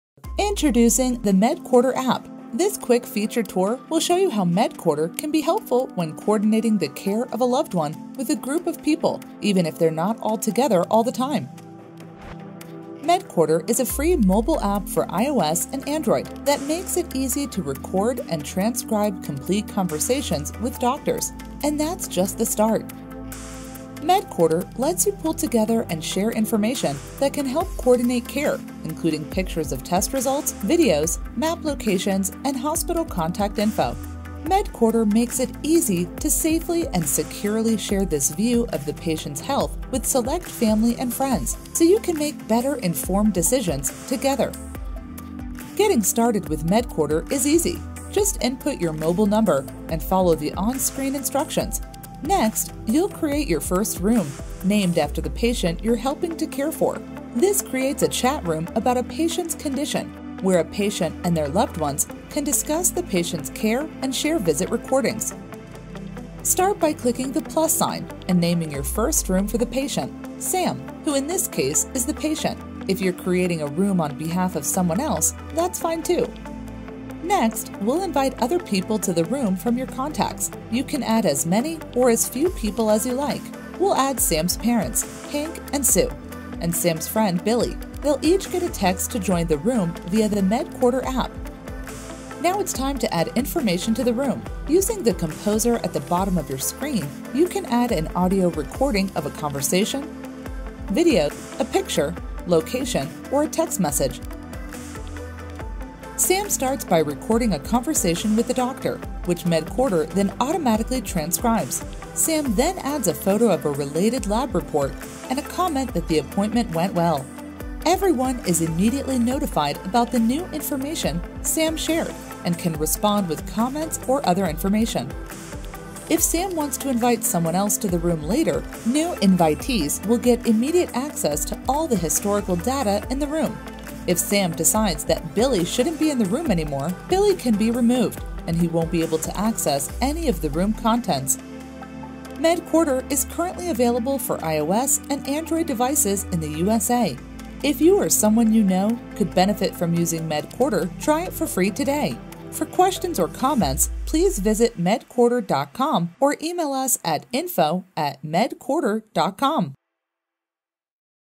Medcorder App Demo
This video will walk you through the functionality of the Medcorder application, which is designed to help patients and their families make the most out of a doctor's appointment by recording the consultation, with the doctor's permission. Recordings are immediately transcribed and shared with the patient's loved ones.

The app is available today for iOS and Android.